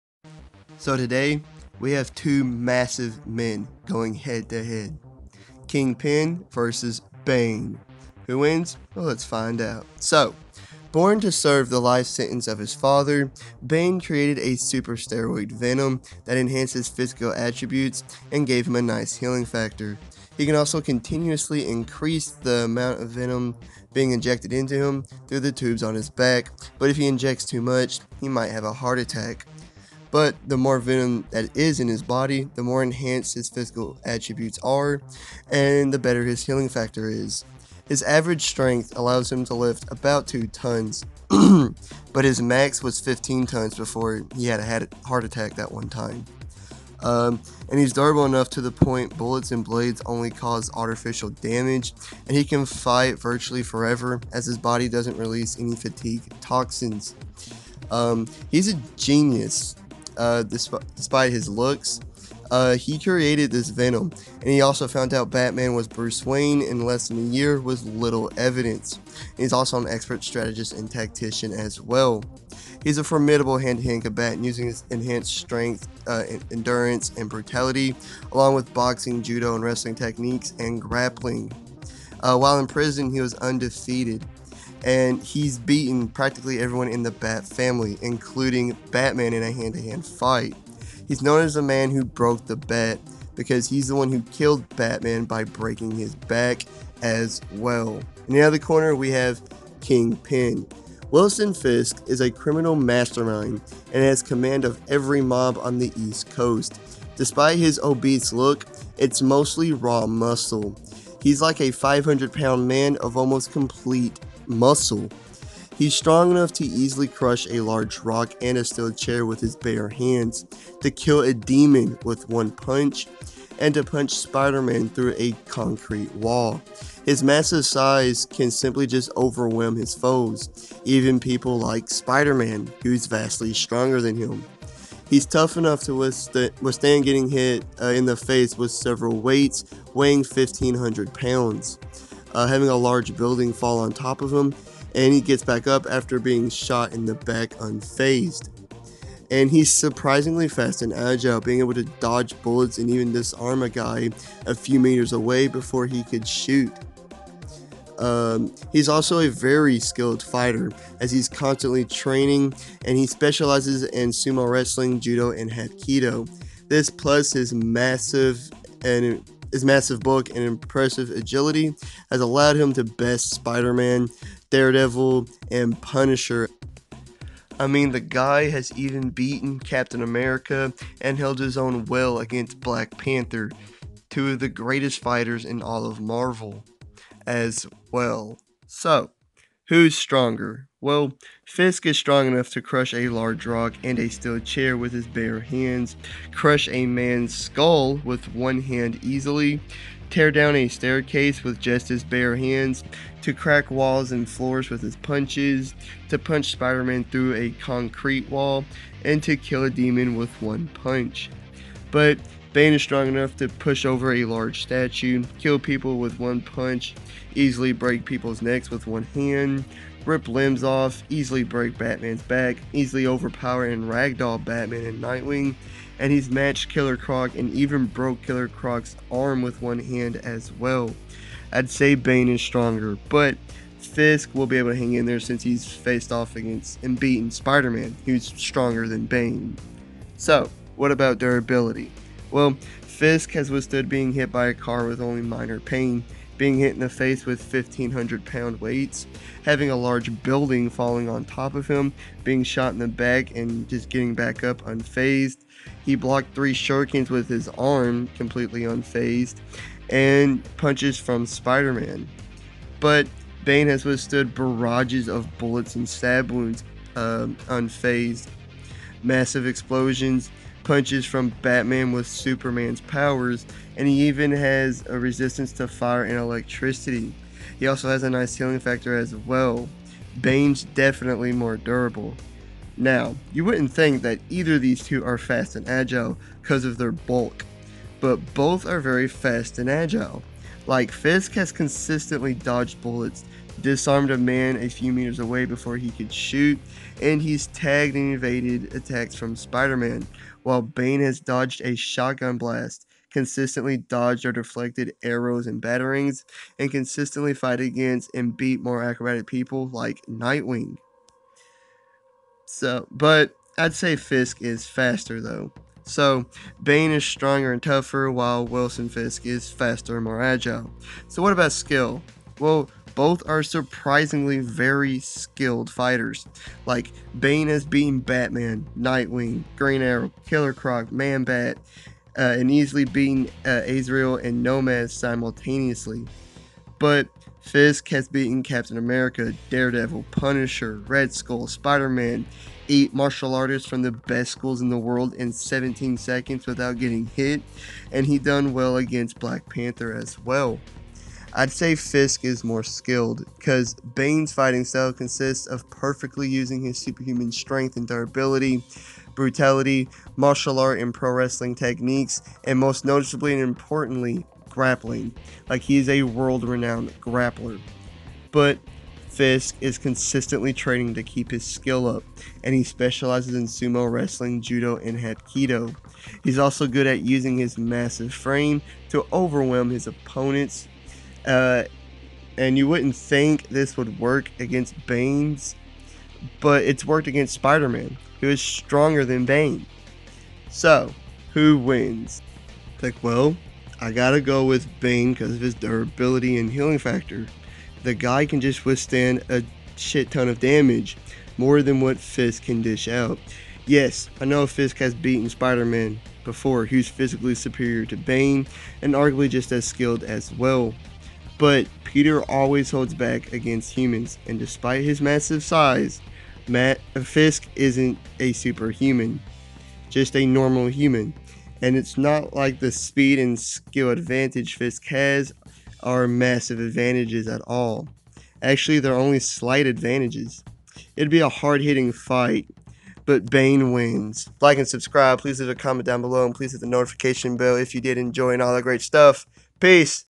Kingpin vs Bane! Who wins? Enjoy!
The Kingpin of all crime vs the Man who broke the Bat! 2 massive men going head to head!
Thanks!!